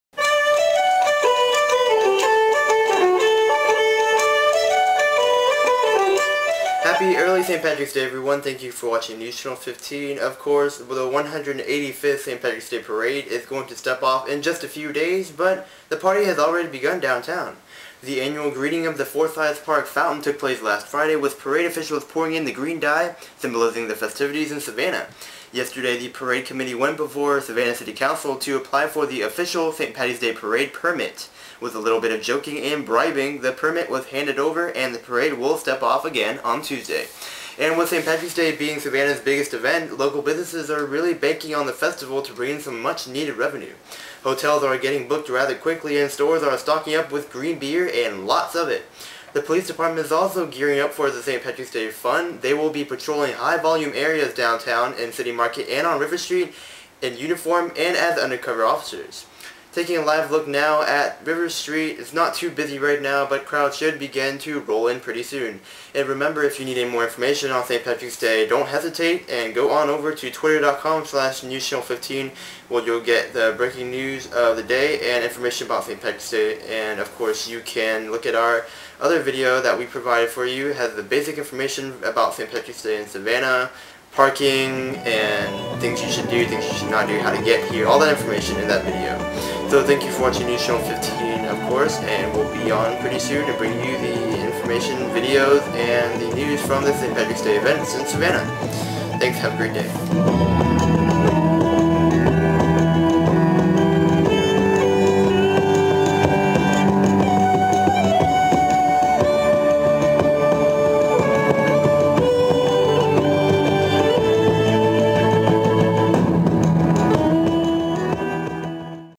Pre-St. Patrick's Day Newscast
The overview of preparations for Savannah's 2009 St. Patrick's Day festivities. From police setting up to the annual "greening of the fountain" in Forsyth Park, things are picking up as the days draw nearer!